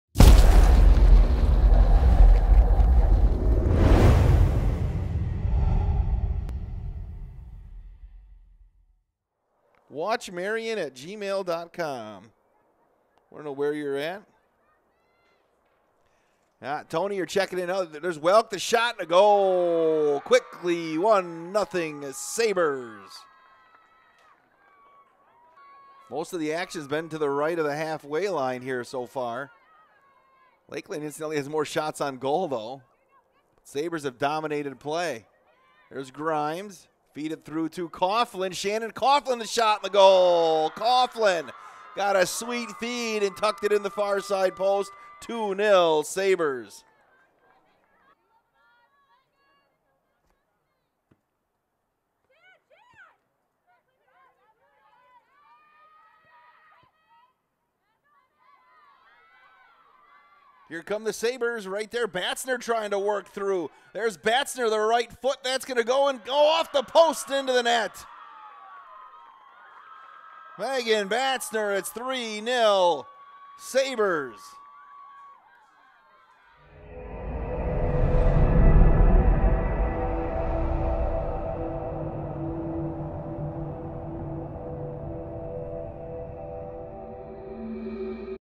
Highlights: Marian Women's Soccer Shuts Out Lakeland
Marian University women's soccer opened a four-match homestand shutting out Lakeland 3-0.